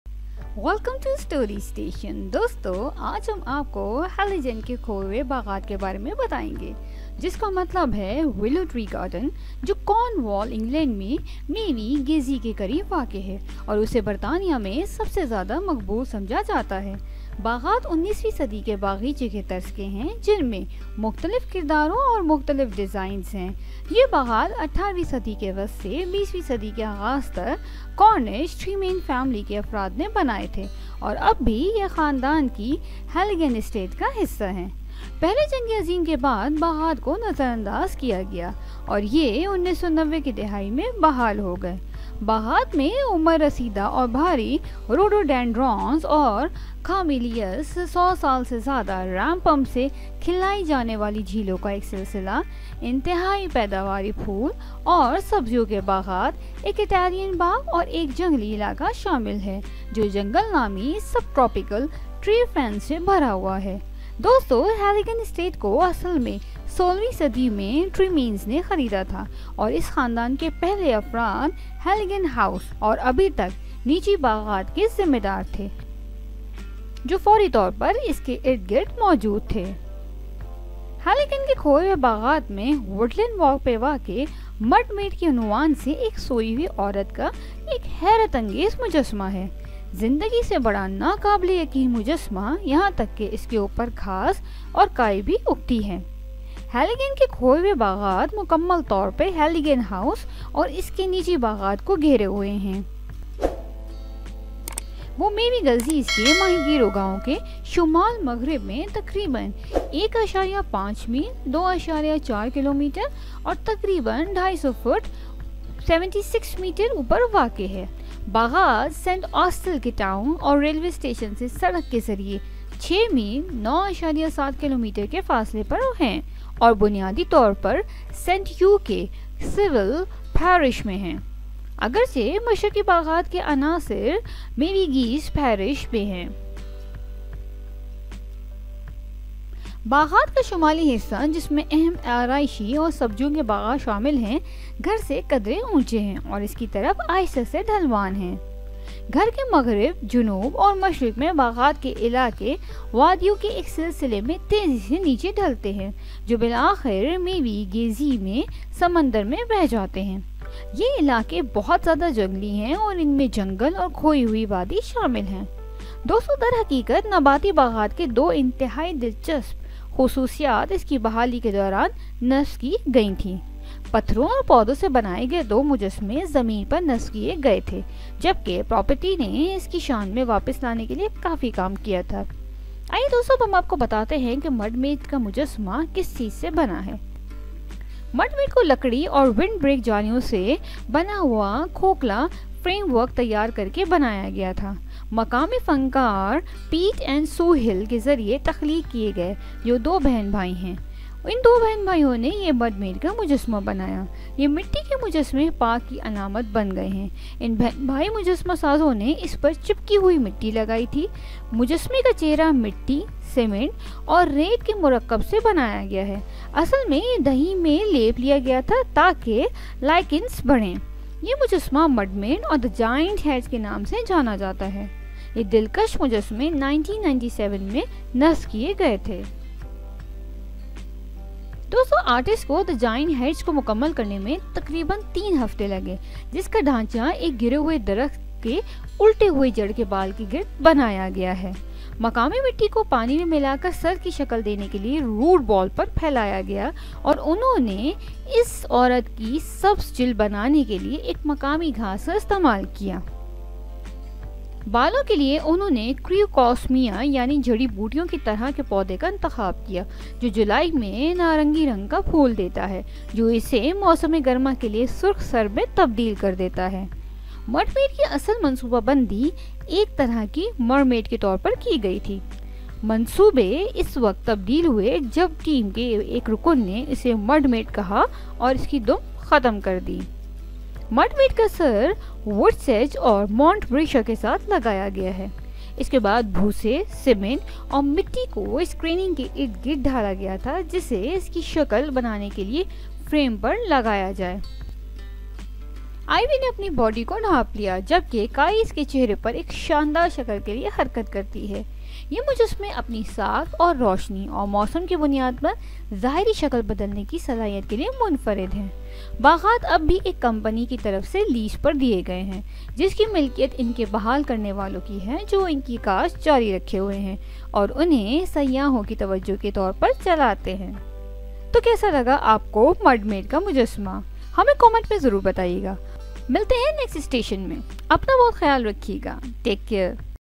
The Sleeping Lady At The Lost Gardens Of Heligan | Story Station
The Lost Gardens of Heligan (Cornish: Lowarth Helygen, meaning "Willow Tree Garden") are located near Mevagissey in Cornwall, England and are considered to be amongst the most popular in the UK. The gardens are typical of the 19th century Gardenesque style with areas of different character and in different design styles.

The gardens were created by members of the Cornish Tremayne family from the mid-18th century to the beginning of the 20th century, and still form part of the family's Heligan estate.

Located on Woodland Walk, in the Lost Gardens of Heligan, is an amazing sculpture of a sleeping goddess entitled ‘Mud Maid’. The incredible larger-than-life sculpture even has grass and moss growing atop it.